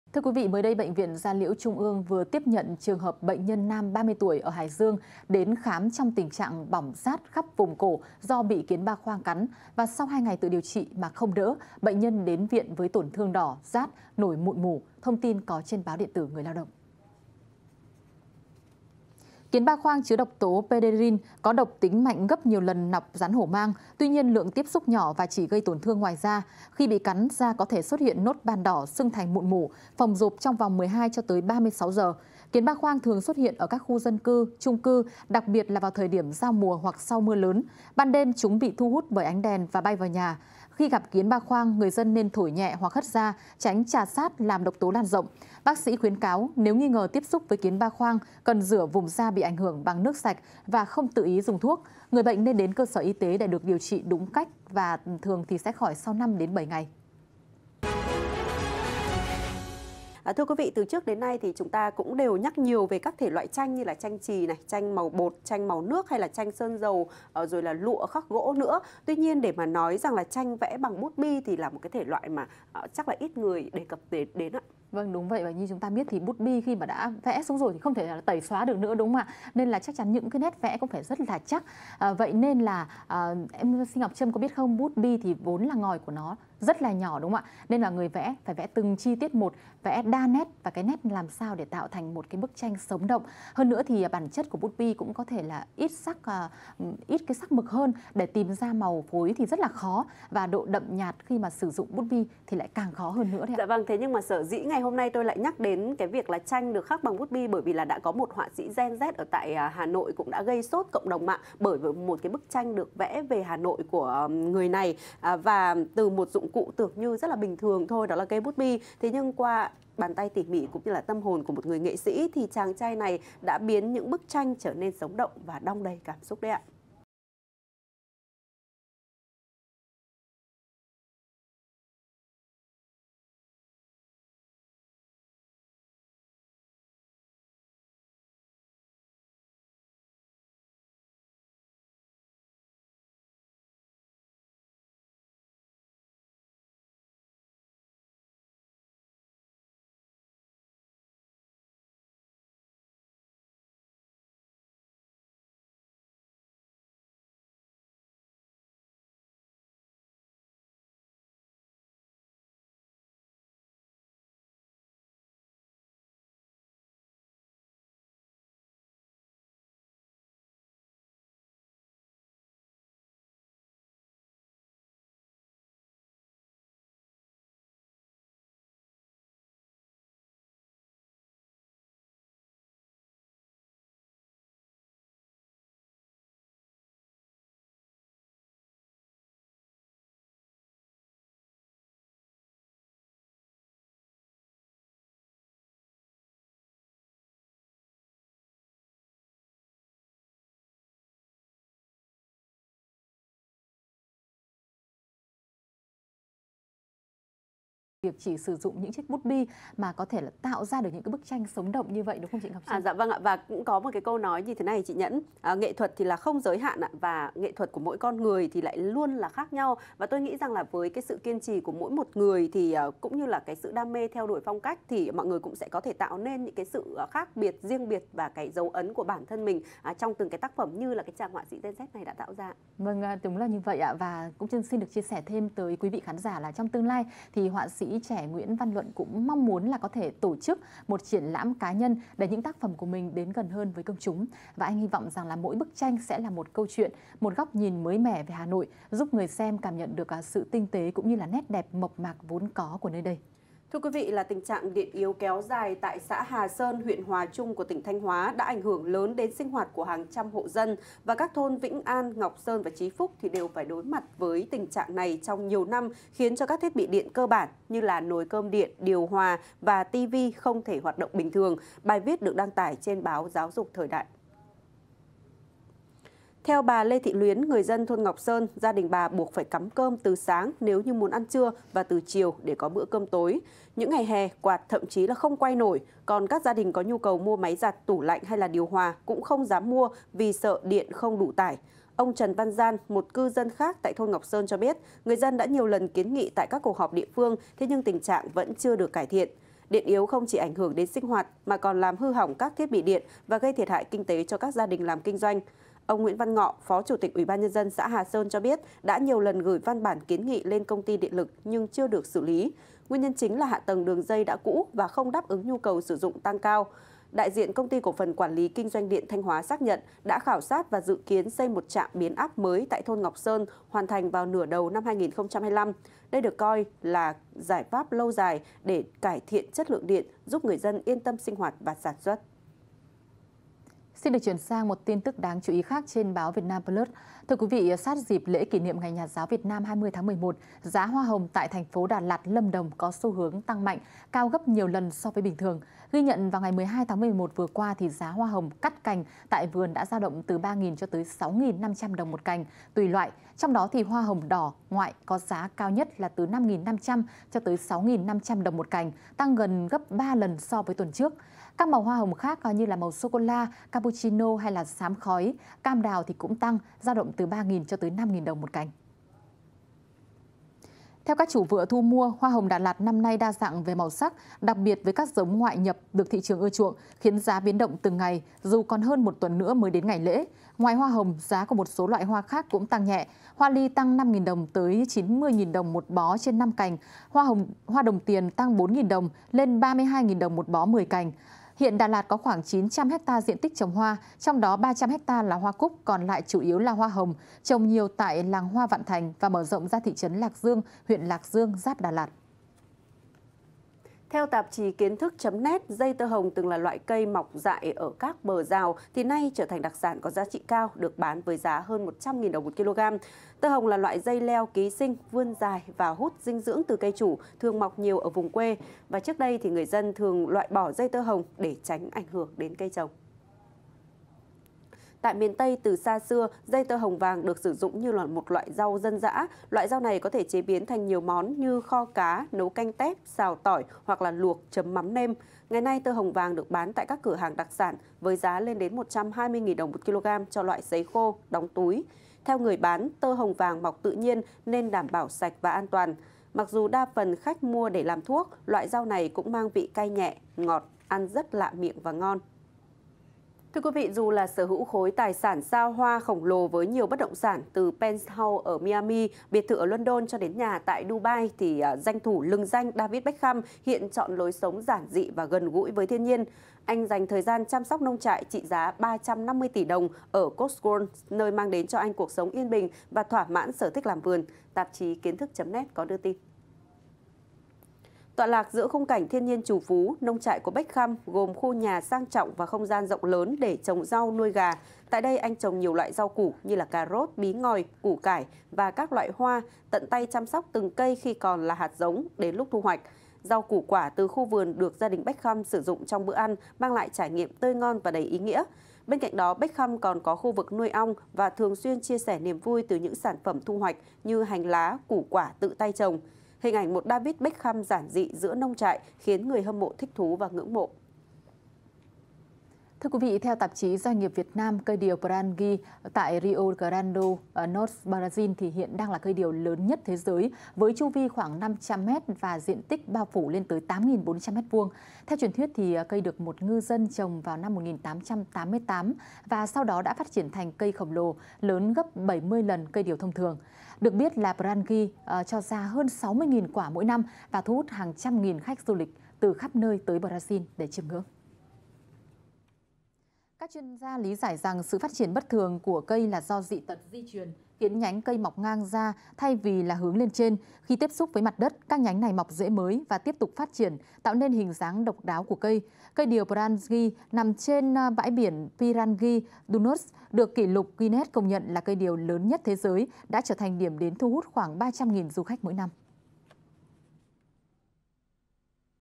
Nhiều người dính độc kiến ba khoang | VTC16
VTC16 | Nhiều người dính độc kiến ba khoang |
Mới đây, Bệnh viện Da liễu Trung ương vừa tiếp nhận trường hợp bệnh nhân nam 30 tuổi ở Hải Dương đến khám trong tình trạng bỏng rát khắp vùng cổ do bị kiến ba khoang cắn. Sau 2 ngày tự điều trị mà không đỡ, bệnh nhân đến viện với tổn thương đỏ, rát, nổi mụn mủ.